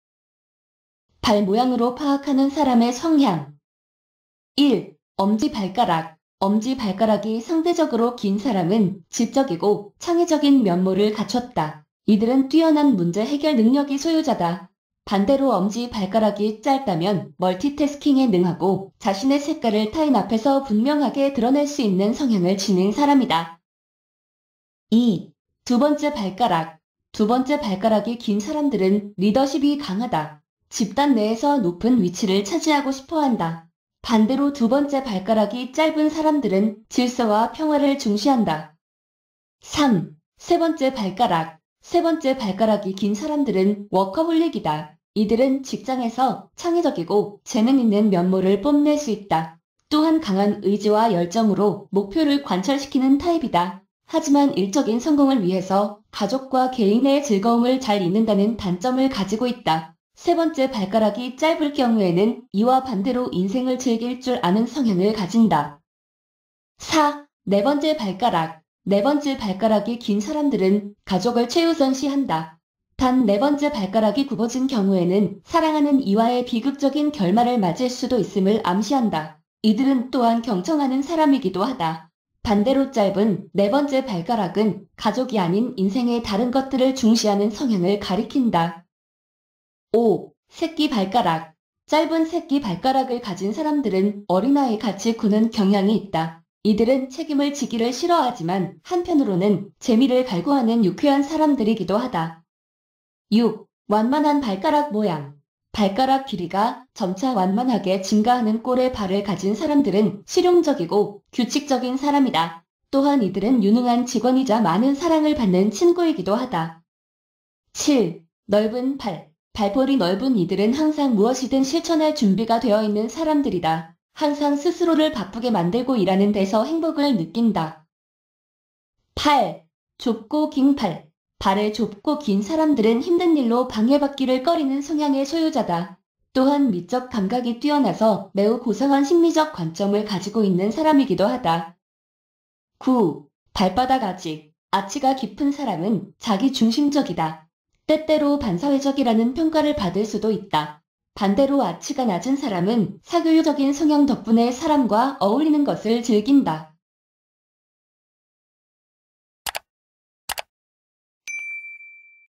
발 모양으로 파악하는 사람의 성향
이러한 모양의 발을 가진 사람은 스포츠를 좋아하는 사람이라고 할 수 있고, 매우 창의적입니다. 새로운 것에 대해 생각하는 것을 좋아합니다.